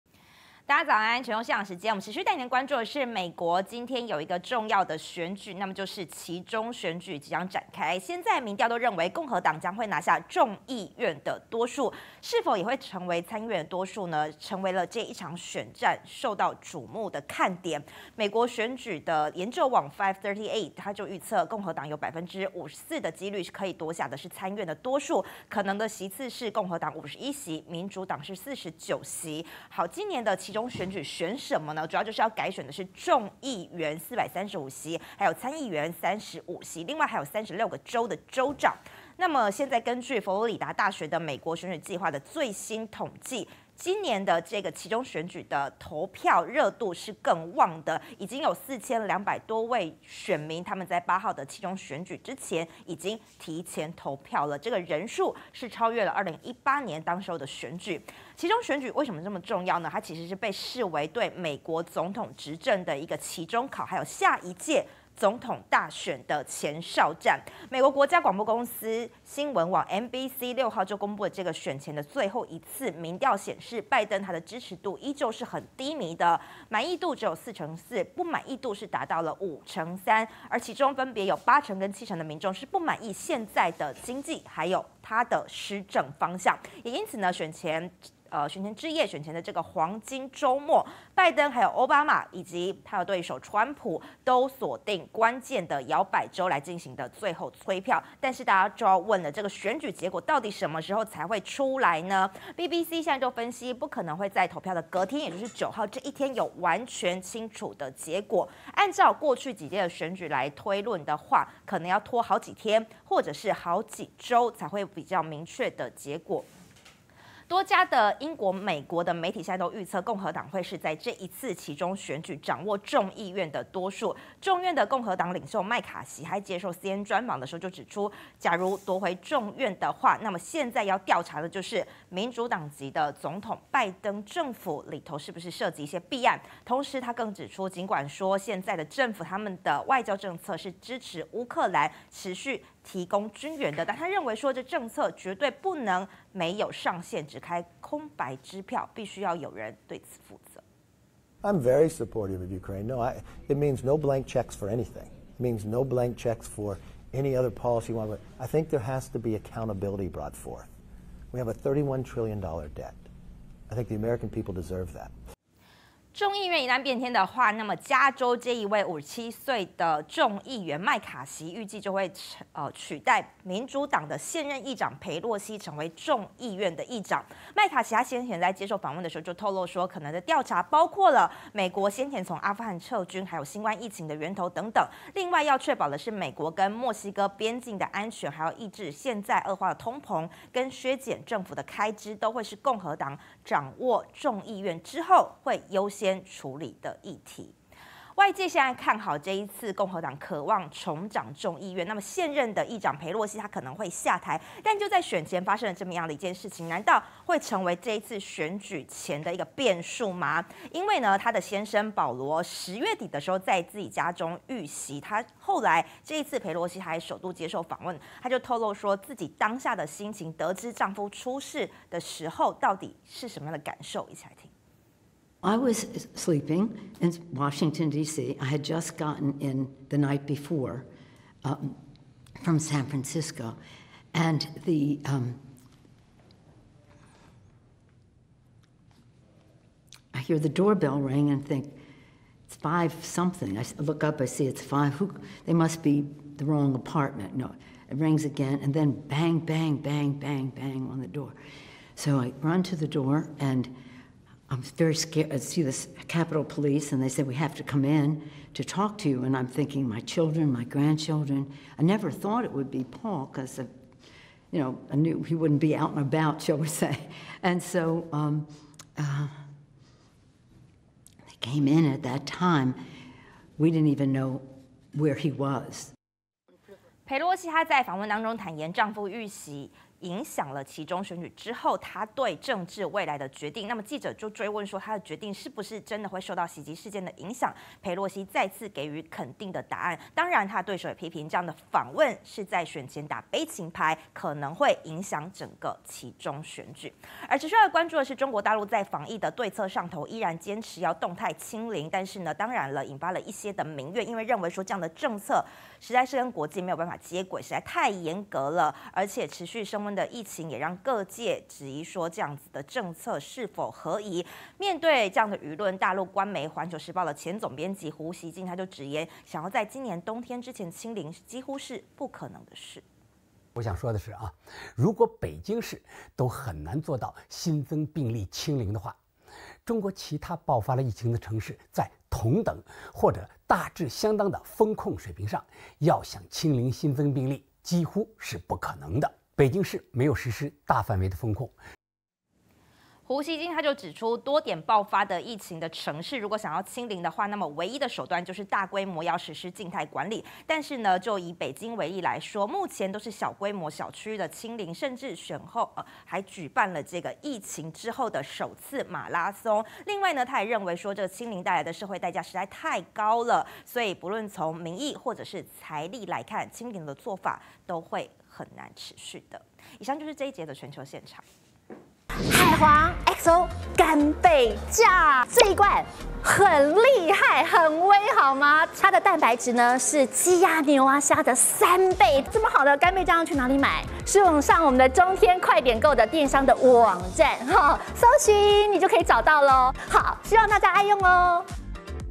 【每日必看】陸疫升溫 胡錫進:清零新增病例幾乎不可能｜丈夫遇襲後首受訪 裴洛西:影響未來政治決定@中天新聞CtiNews 20221108
00:00 美期中選舉若變天 共和黨領袖:調查拜登政府
03:19 丈夫遇襲後首受訪 裴洛西:影響未來政治決定
04:58 陸疫升溫 胡錫進:清零新增病例幾乎不可能